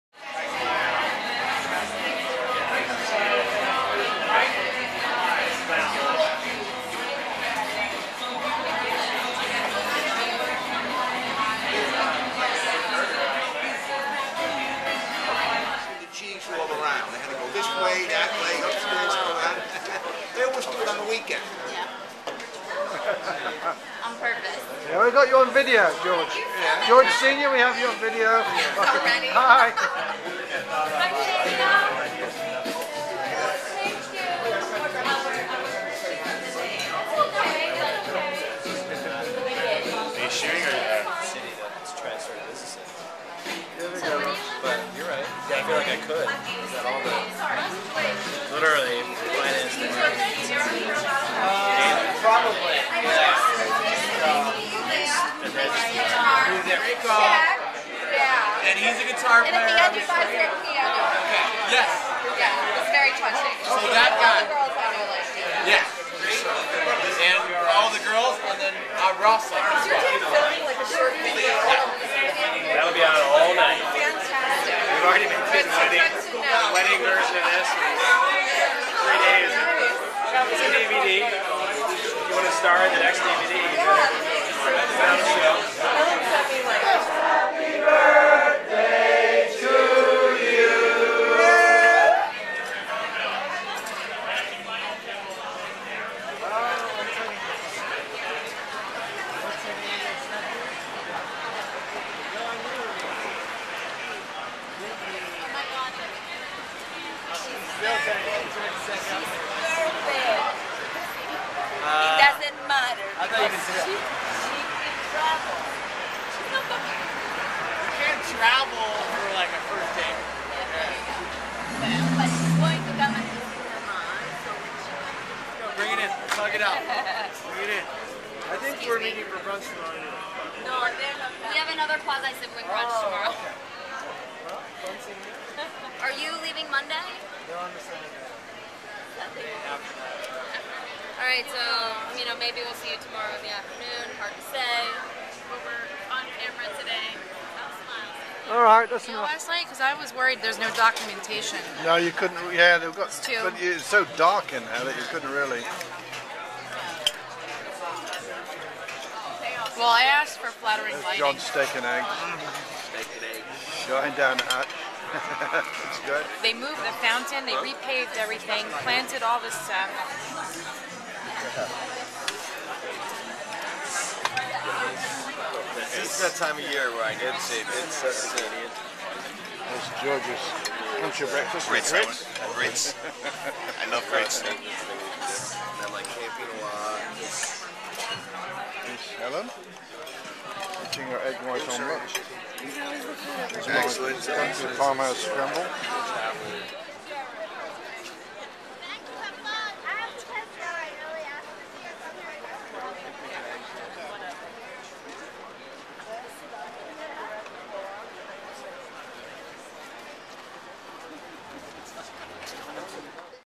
Helen's 40th birthday weekend
Some clips from Helen's birthday party organized by George at an Irish pub in SoHo NYC + day after